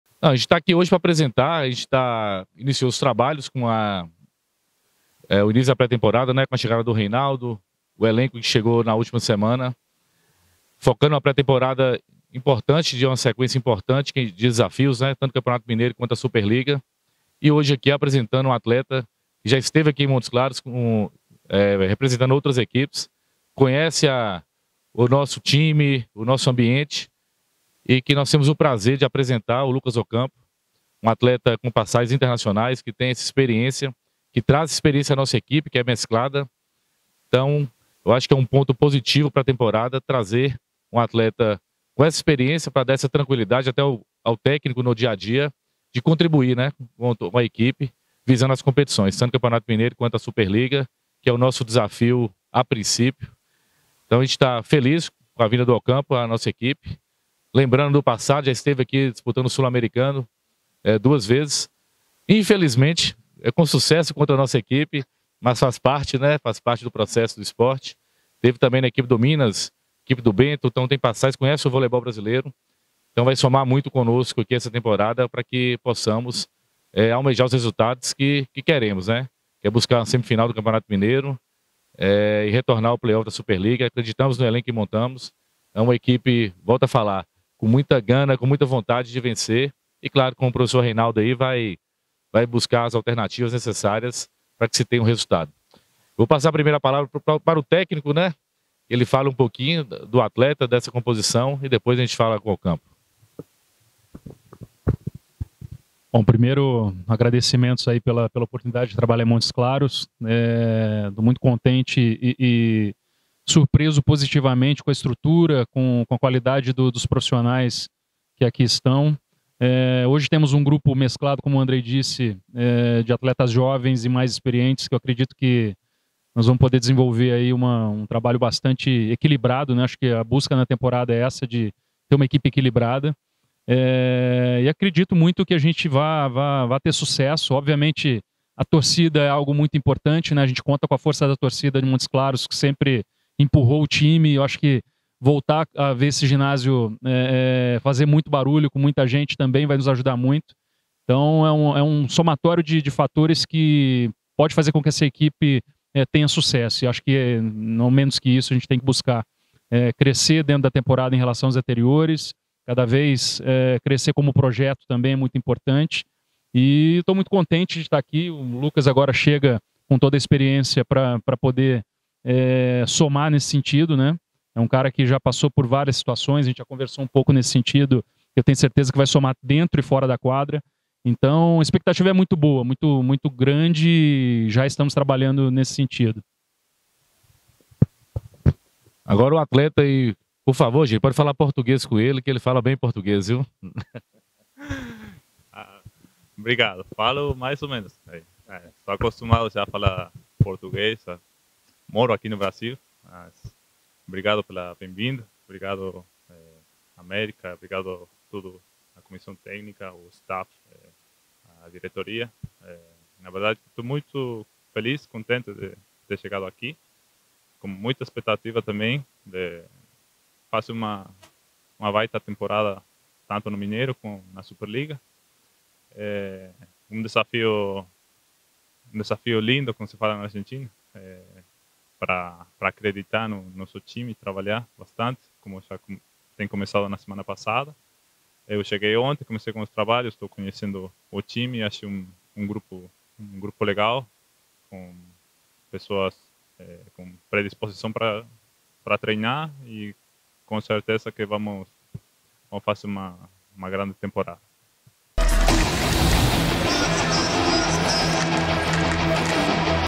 COLETIVA DE IMPRENSA | APRESENTAÇÃO DO PONTEIRO ARGENTINO LUCAS OCAMPO
Confira na íntegra a coletiva de imprensa com o ponteiro Lucas Ocampo